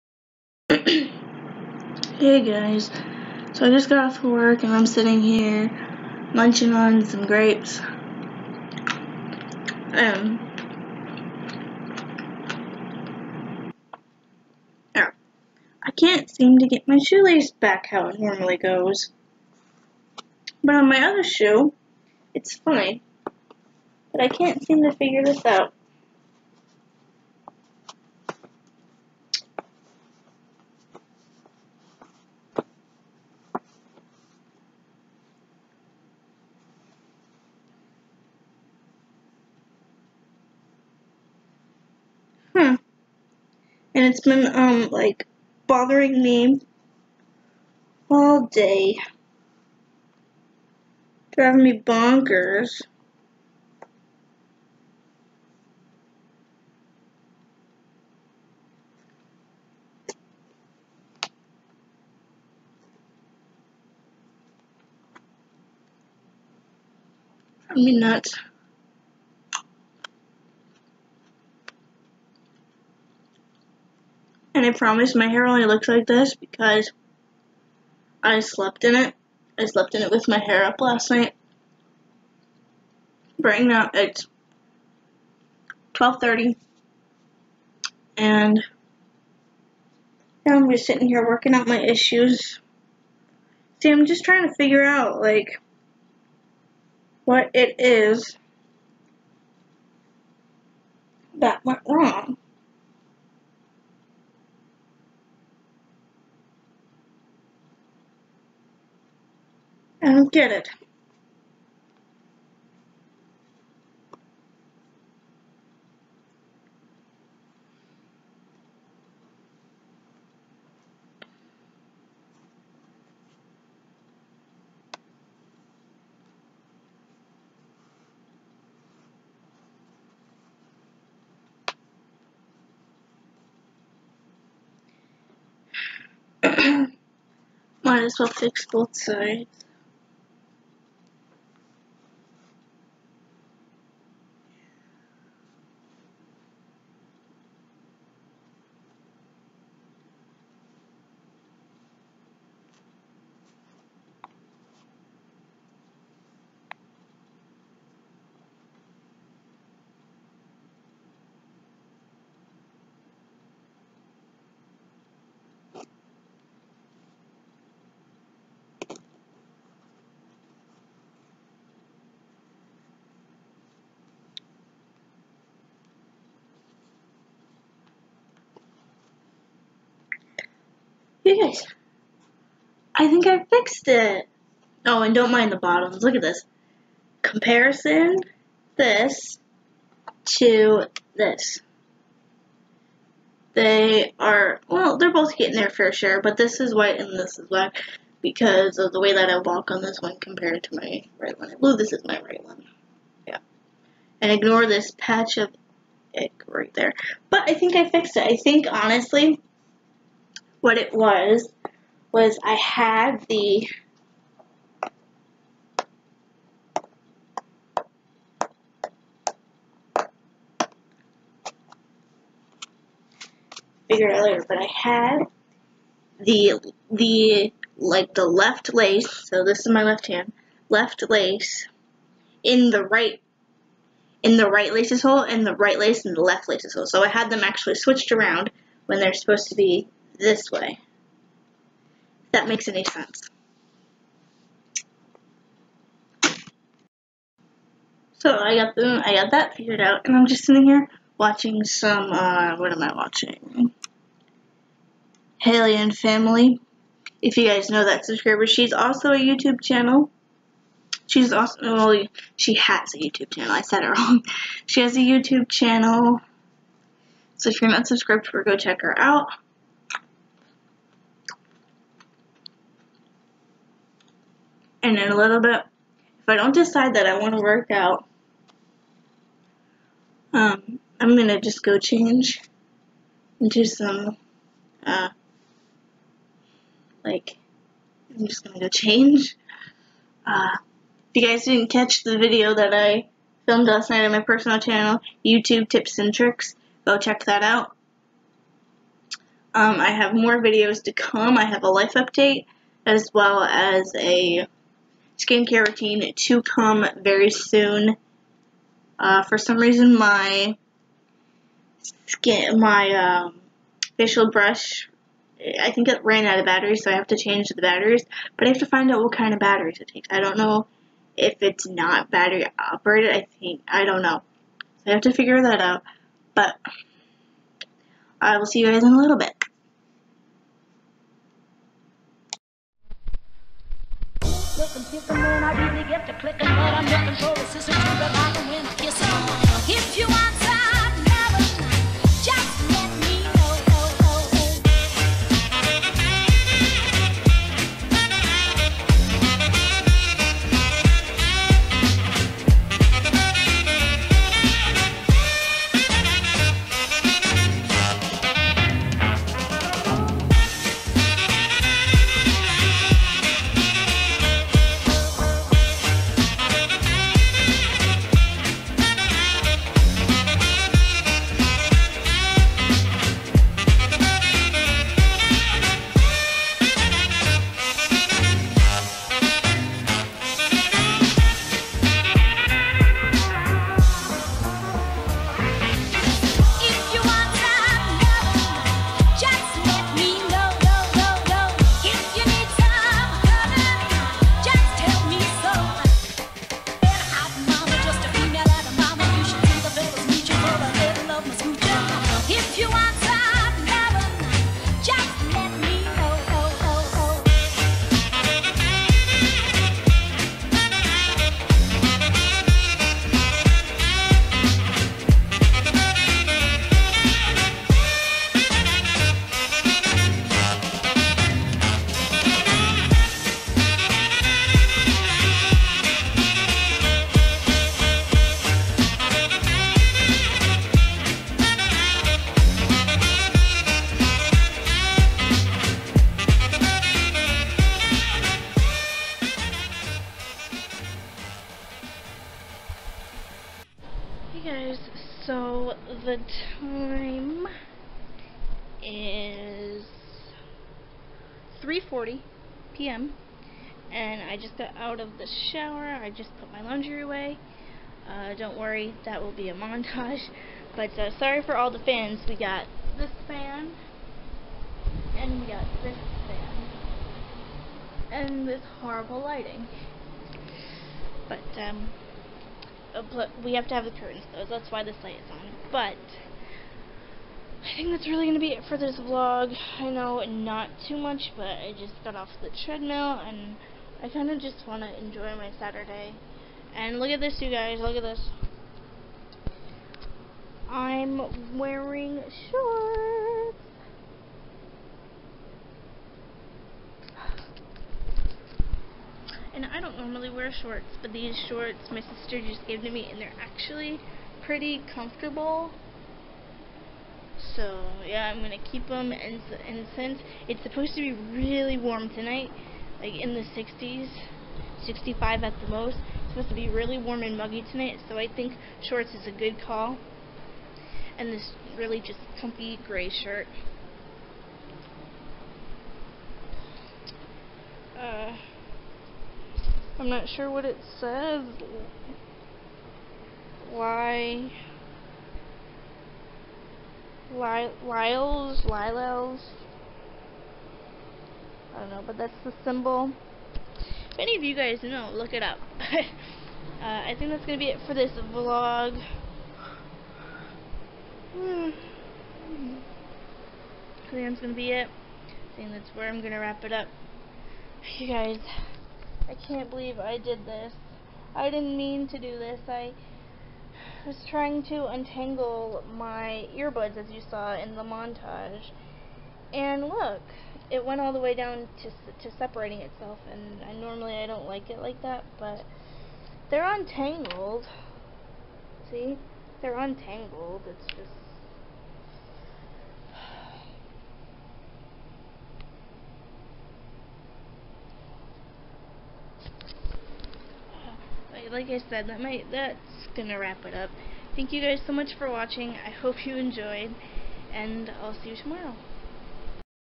Hate When This Happens
It is almost ninety degrees, if it is not already. I eat lots of grapes and have problems with my sneakers. Keep watching to find out more. Hope you enjoy.